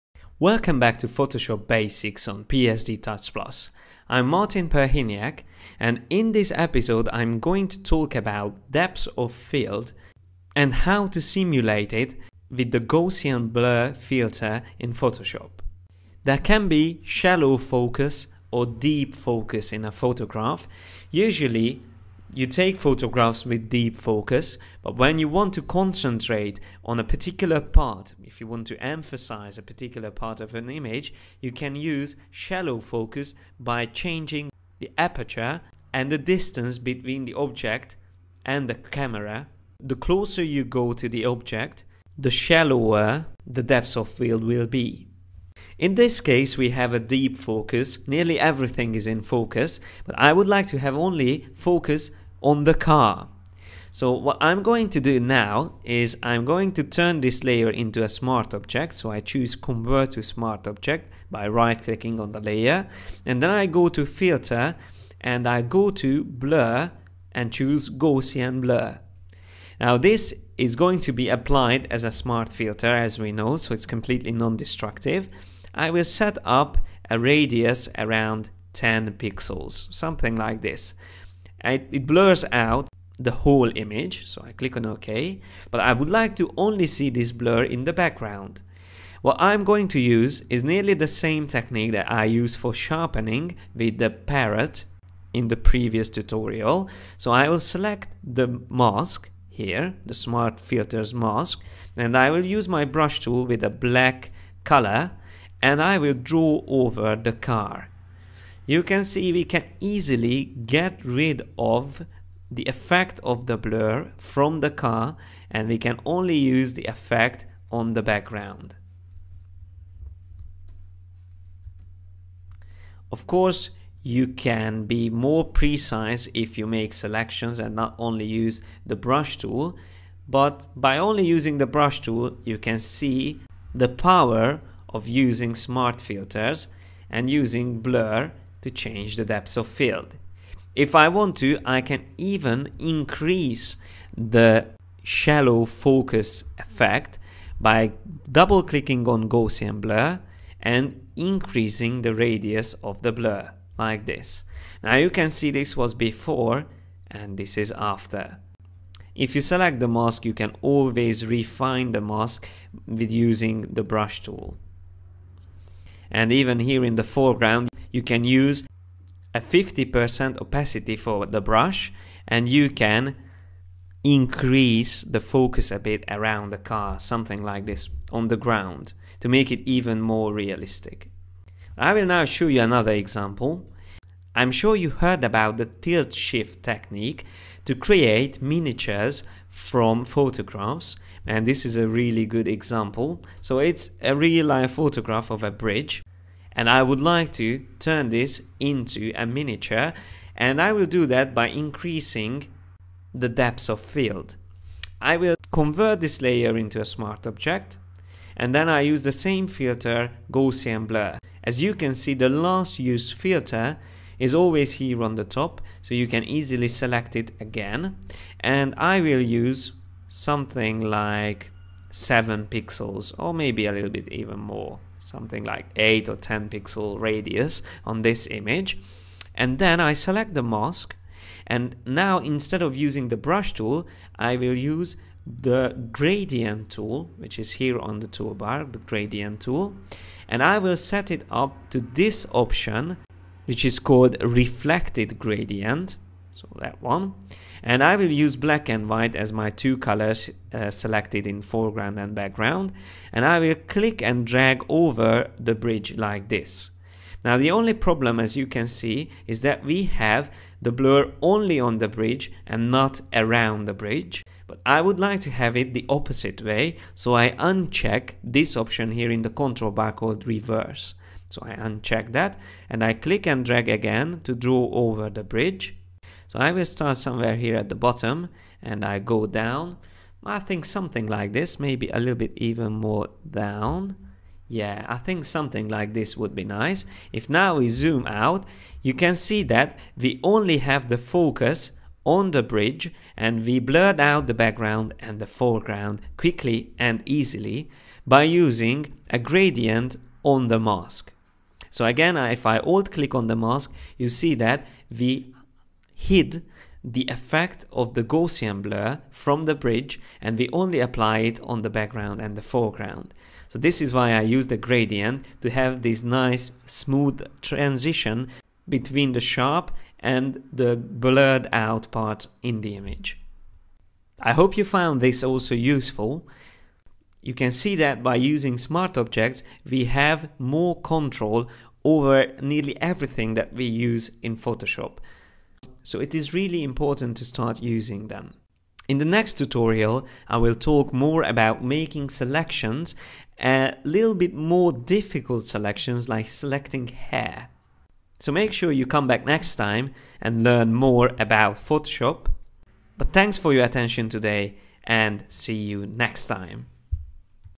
Depth of Field is What You Need
In this tutorial, we will explain how to apply Gaussian blur as a smart filter to simulate shallow focus in Photoshop.